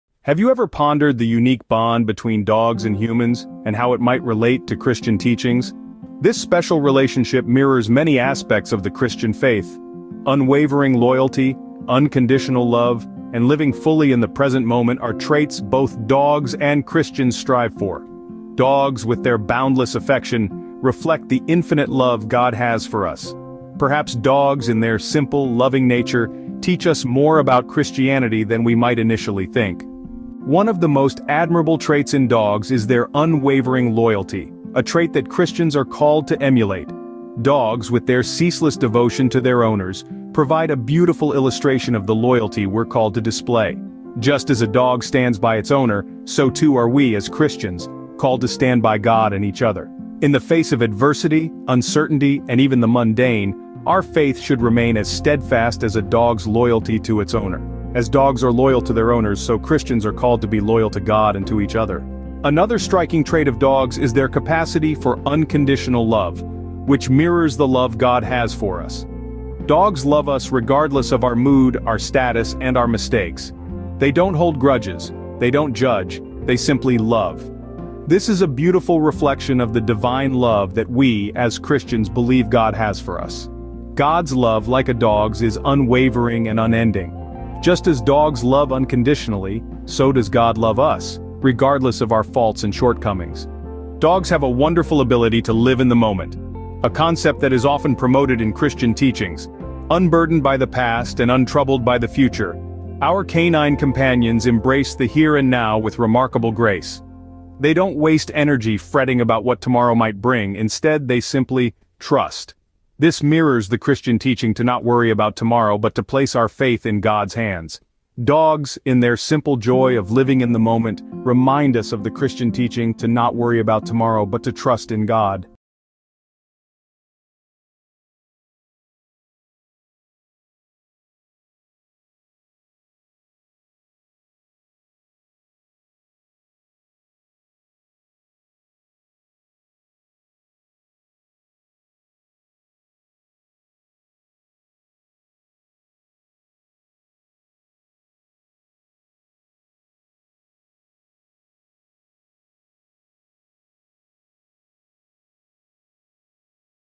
Beautiful lessons every Christian should learn from dogs
In this heartwarming video, we explore the incredible qualities of our furry friends and how they can inspire Christians to live a more fulfilling and Christ-like life. From loyalty to unconditional love, dogs can teach us valuable lessons about faith, compassion, and living in the present moment. Join us on this journey of self-discovery and see how our four-legged companions can guide us on the path of spiritual growth.